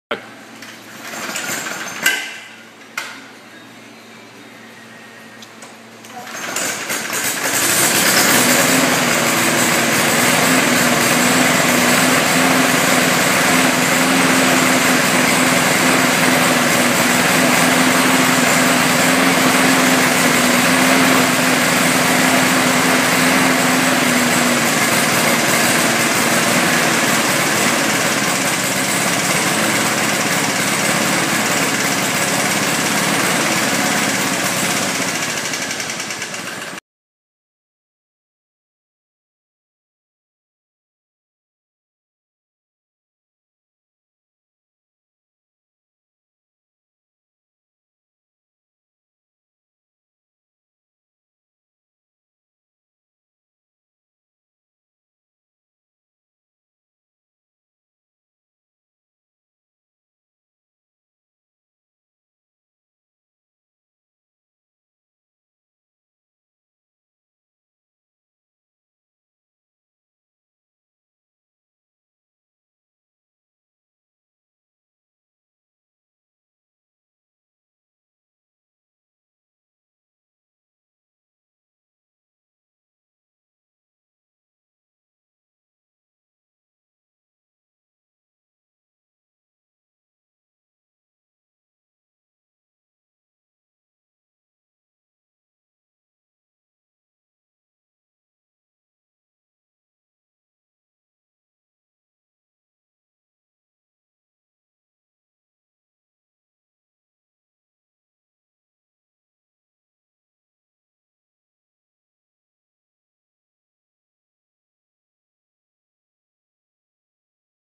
Briggs and stratton broken flywheel key symptoms and repair
This is a quick video to show how a Briggs engine runs when the flywheel key breaks after the engine is stopped suddenly after hitting a solid object. The flywheel kept rotating for about 45 degrees. The repair is cheap and easy. Remove the flywheel and replace the key. To repair the broken flywheel key, first remove the flywheel retaining nut. You will need to stop the crank from turning when loosening the nut. I used a piece of timber between the blade disk and the body. Replace the nut to protect the thread (in case you miss with the hammer) and get a lever under the fly wheel and apply some upwards pressure. Give the top of the crank shaft a sharp crack with a hammer and the flywheel should release. Remove the broken flywheel key and replace the flywheel. Insert a new flywheel key, replace the nut and tighten. Replace the engine cover and starter and the job is done.